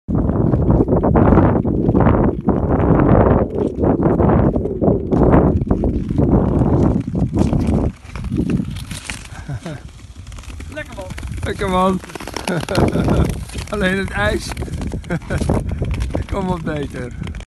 Elfstedentocht 2021 - OutdoorXL
In elke goede vorstperiode met ijs, is het één van de belangrijke vragen. Gaat hij gereden worden, de Elfstedentocht. De winter van 2021 was in ieder geval zo goed, dat er pogingen zijn gedaan.

Henk Angenent ging onderweg op 14 februari en strandde in Bolsward. Maar er ging ook een OutdoorXL team op pad. Nou ja, OutdoorXL-team… de 2 OutdoorXL’ers  konden niet mee. Die waren zondagochtend nog schaatsen aan het verkopen.

Maar de Friese familie Rusticus ging wél onderweg. En de voorbereiding is natuurlijk goed als je bij pap en mam in Leeuwarden kan slapen en naar het startpunt op de Zwette wordt gebracht. Om 5:35 uur kregen we het eerste appje: over 15 minuten starten we. Het weer was goed en de volgauto met reservekleding, warme chocolademelk en support was er ook. 5:50 uur: It giet oan, Edwin, Sven en Hans gaan onderweg!